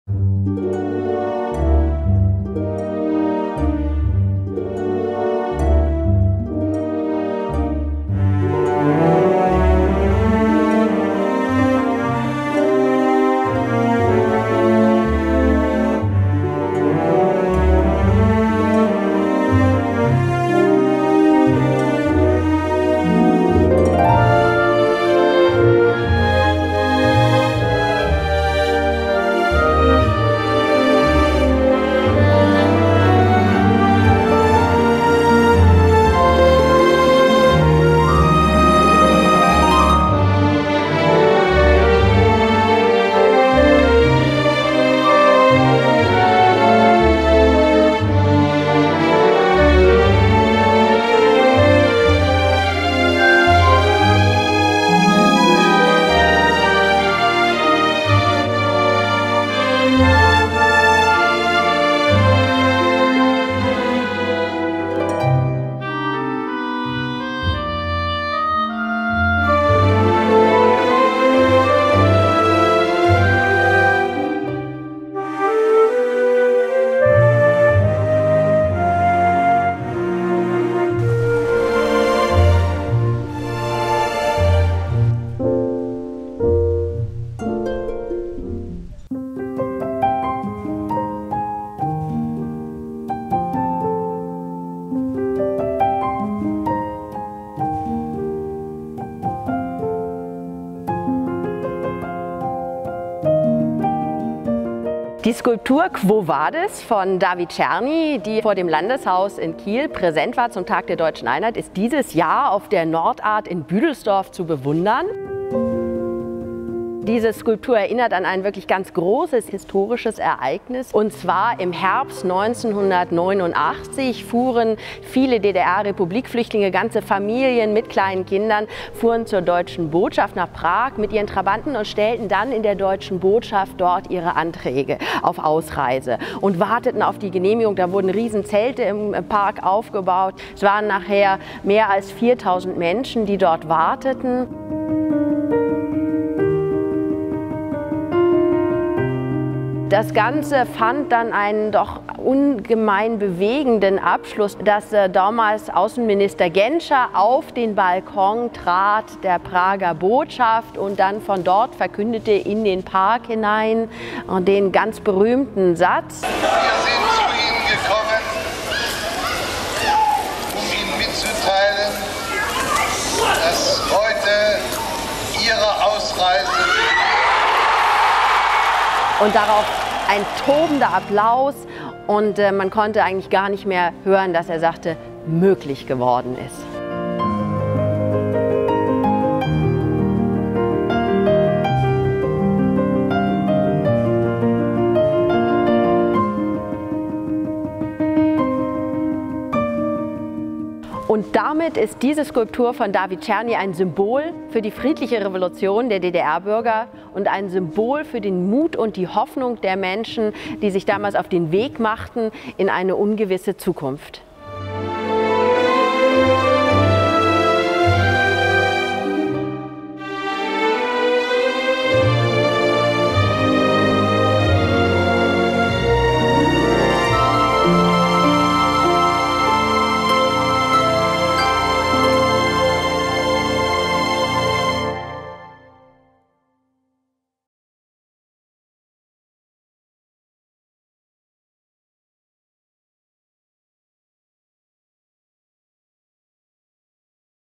"Quo Vadis" by David Černý - NordArt 2019
Der zwölfte Film aus unserer Filmreihe über die NordArt 2019 (Geöffnet: 1. Juni bis 13. Oktober 2019).
"Quo Vadis" heißt die Skulptur von David Černý, die dieses Jahr auf der NordArt 2019 in Büdelsdorf zu sehen ist. Eine Skulptur mit einem historischen Hintergrund, der dafür sorgte, dass sie am Tag der Deutschen Einheit 2019 auch in Kiel vor dem Schleswig-Holsteinischen Landtag zu bewundern war.

"Quo Vadis" is the name of the sculpture by David Černý, which can be seen this year at NordArt 2019 in Büdelsdorf. A sculpture with a historical background, which made sure that on the Day of German Unity in 2019 it was also to admire in Kiel in front of the Schleswig-Holstein Landtag.

NordArt
Vorwerksallee
24782 Büdelsdorf